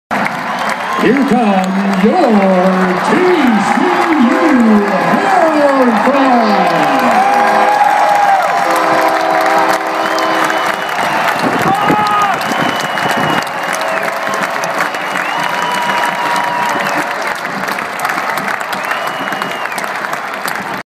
TCU takes the field in the new Amon Carter Stadium
The TCU Horned Frogs football team take the field for the first time at the new Amon G Carter Stadium on Saturday September 8th, 2012 against Grambling State University.